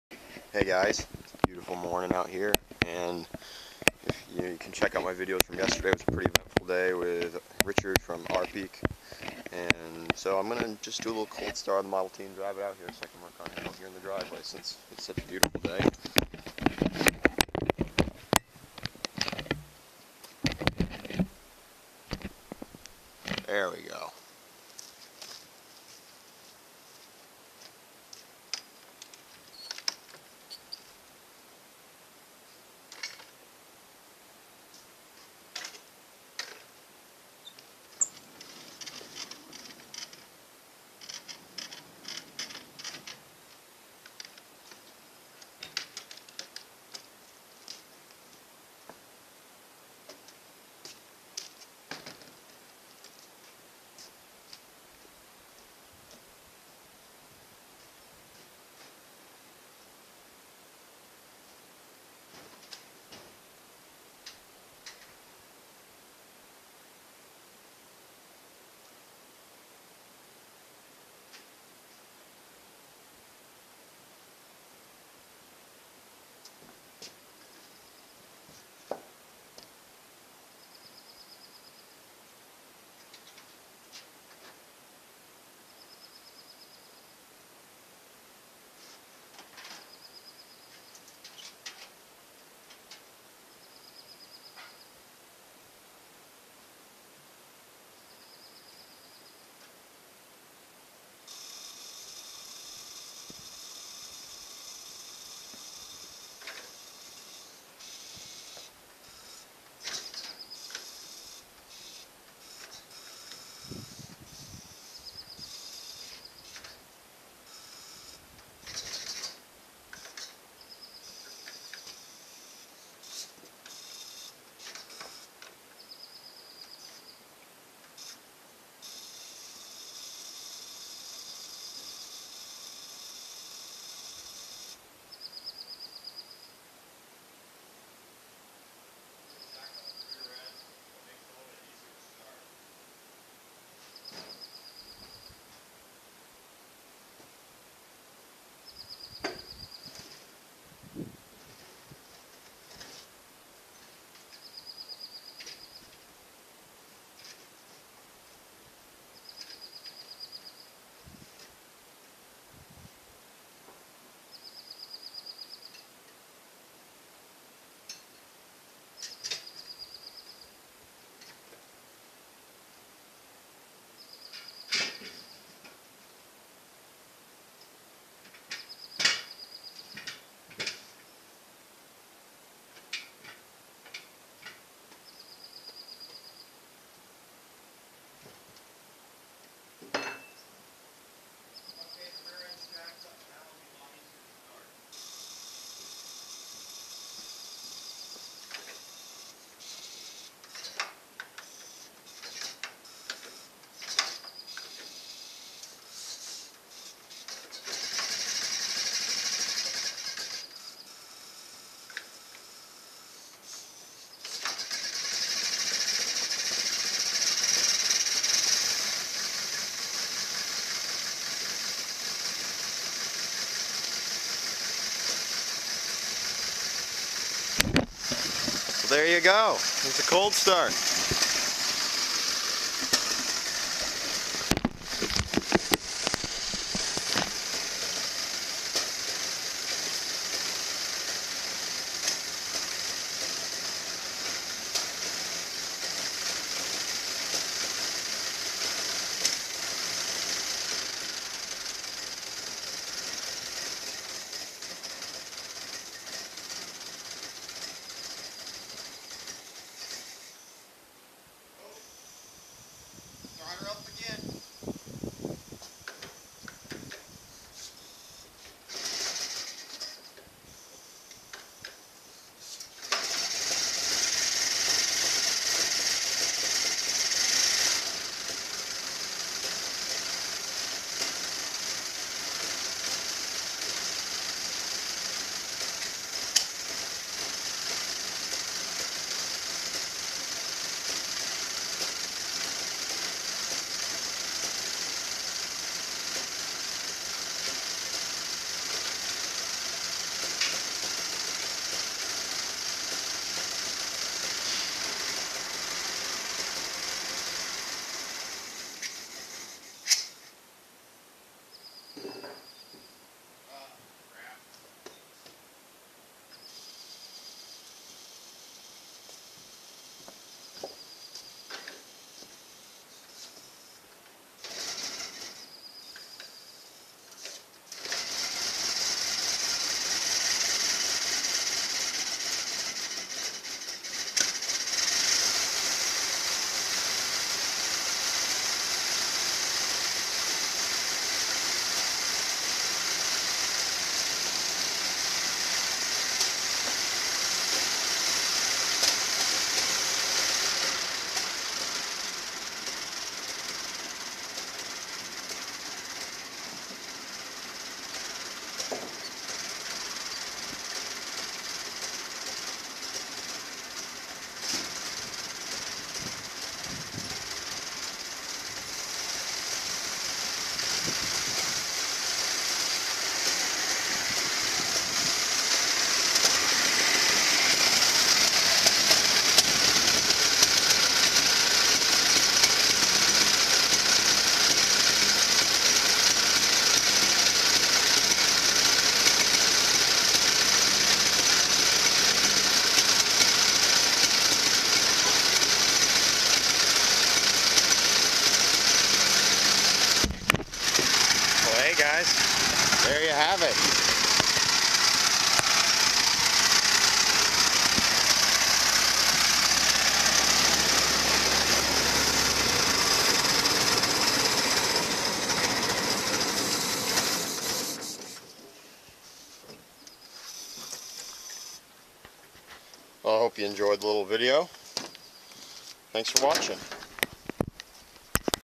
Cold start of the 1926 Ford Model T Tudor sedan
Just thought I would do a cold start.